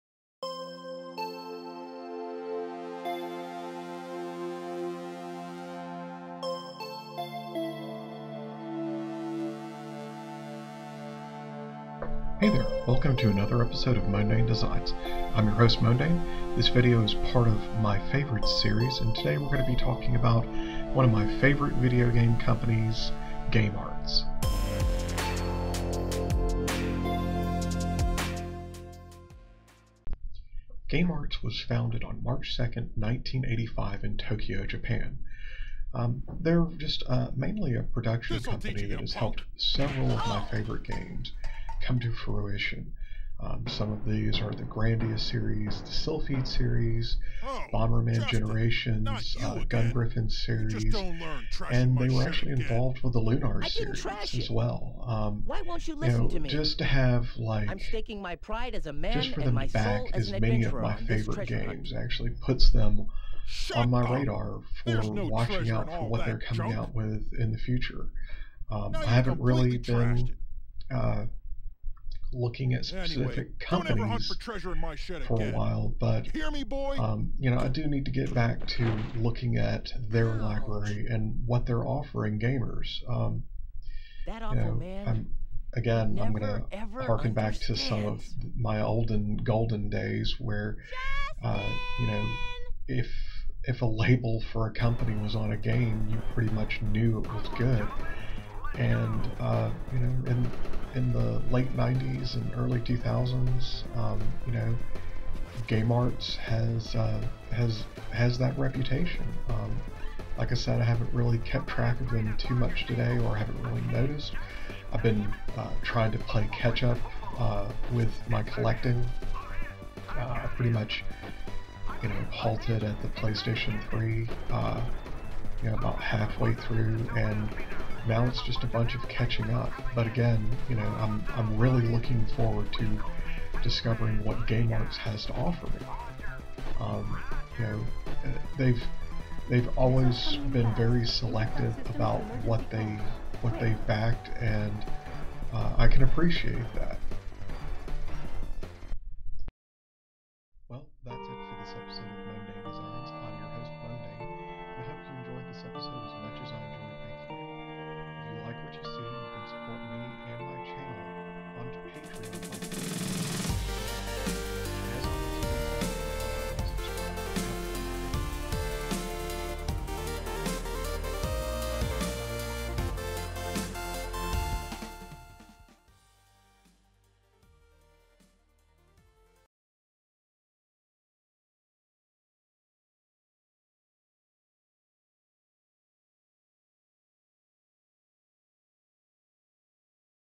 GameArts Video game Company review
Game Arts is one of my favorite game companies. You should check them out. With a great library of game spanning a large number of genres, you should be able to find something to play. What Game Arts games do you own?

Special Thanks to: Twilights Dawn